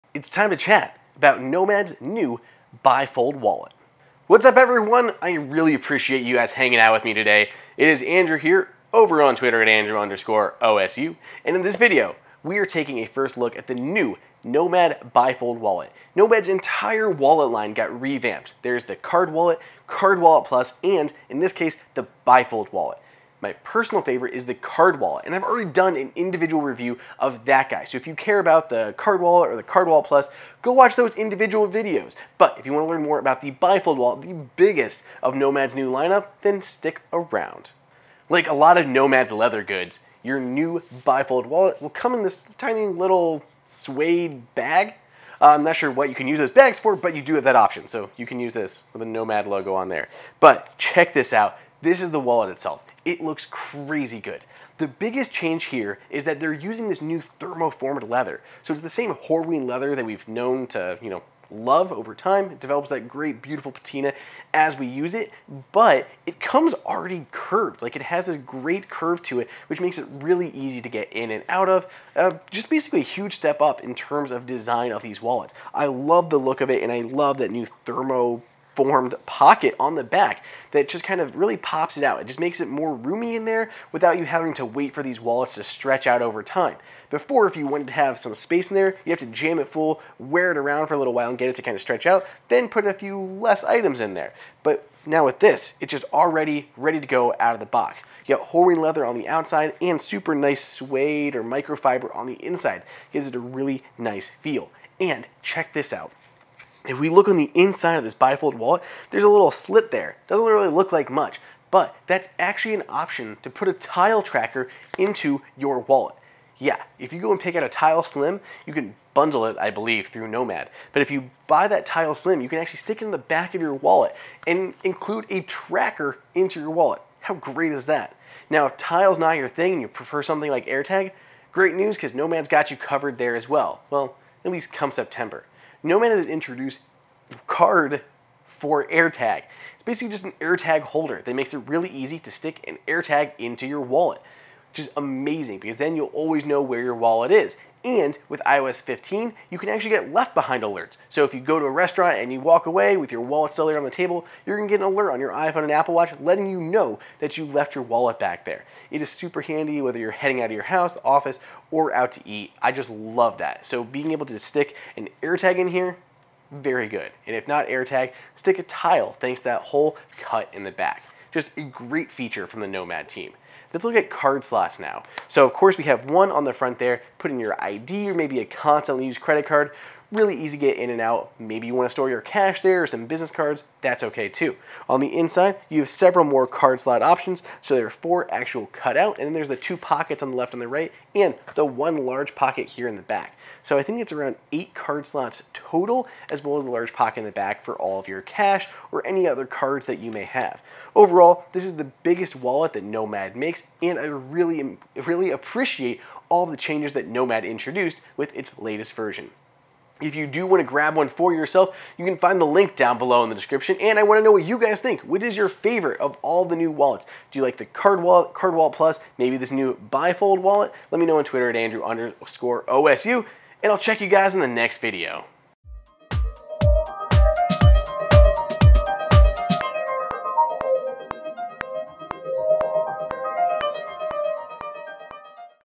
Review: Nomad's Bifold Wallet Will Hold All Your Essentials
If you want the most capable wallet from Nomad then you’re looking for the new Nomad Bifold Wallet. It’s thermoformed Horween Leather can hold more than 14 cards as well as cash without issue.